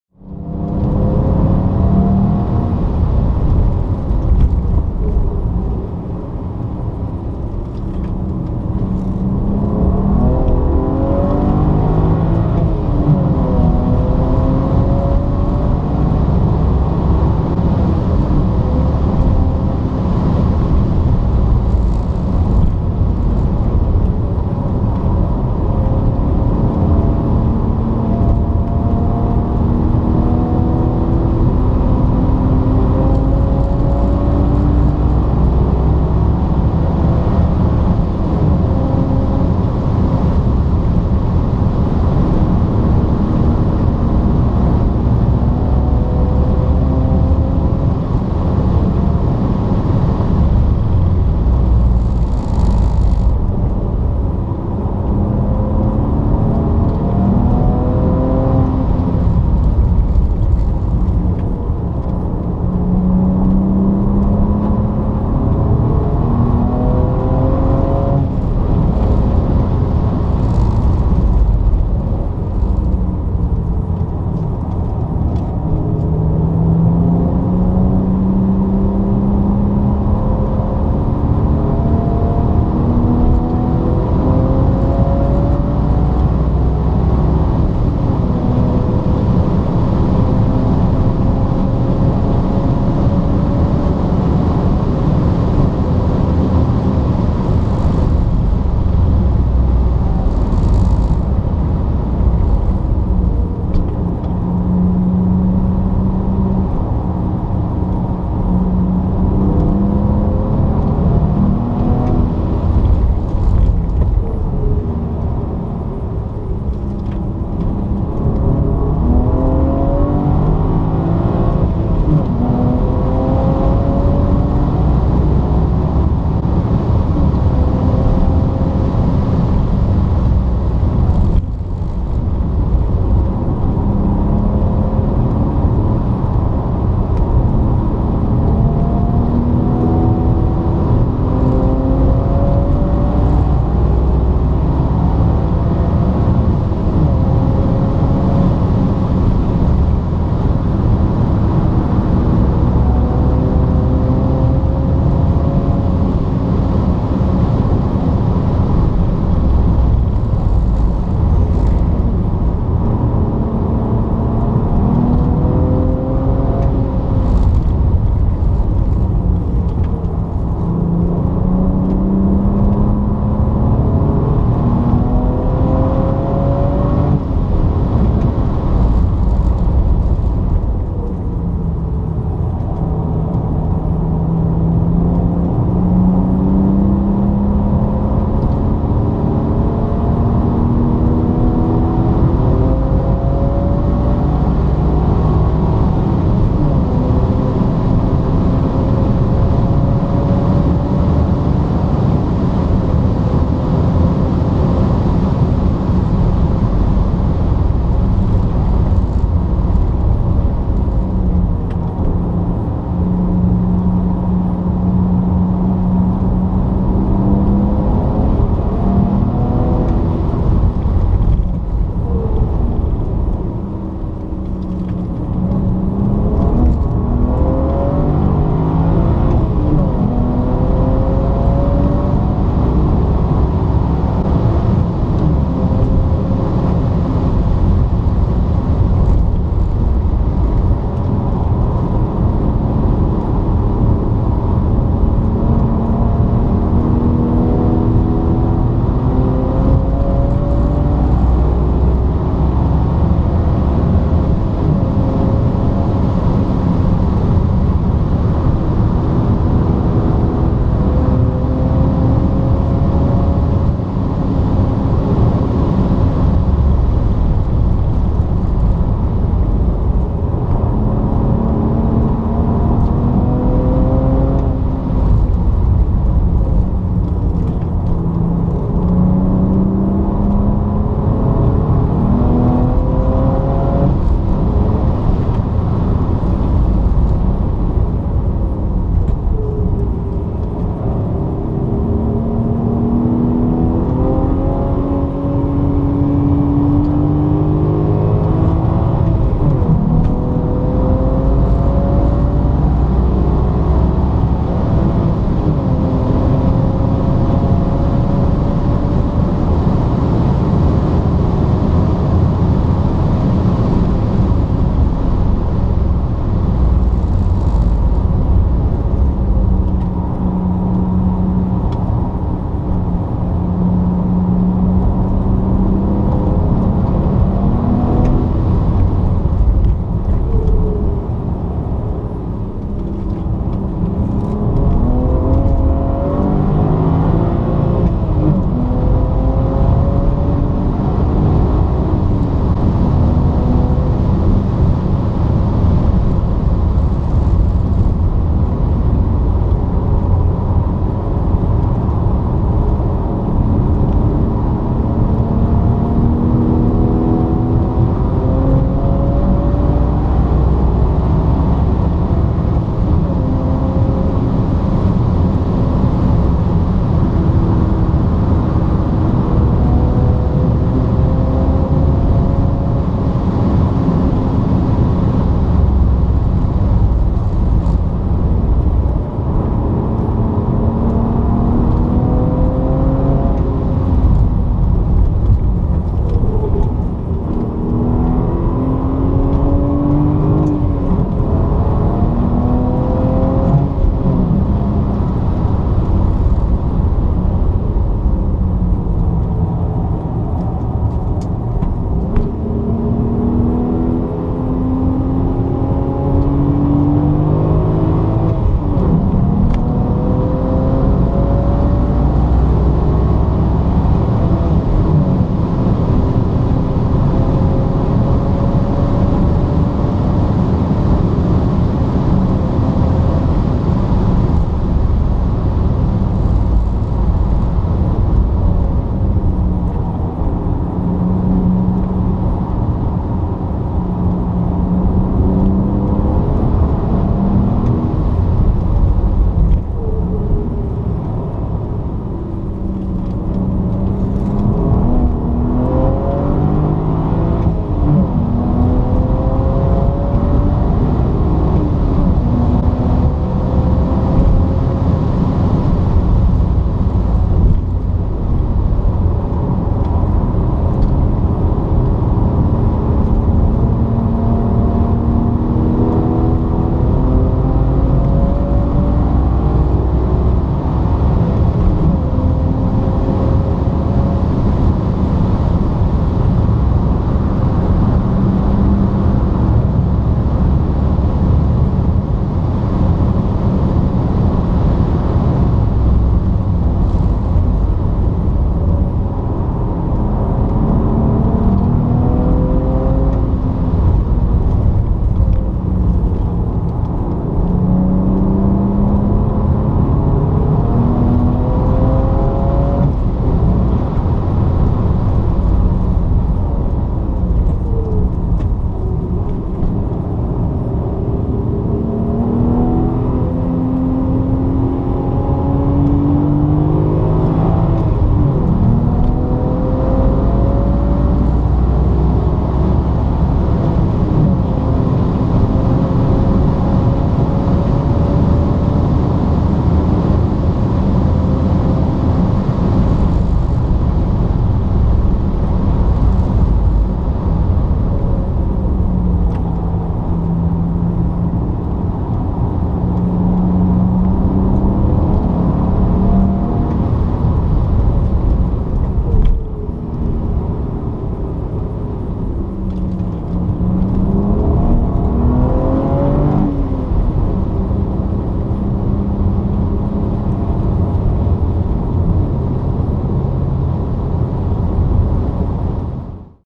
Goodwood May 2014 - Session 4
Session 4, track finally dry but still being cautious into the breaking with the new car!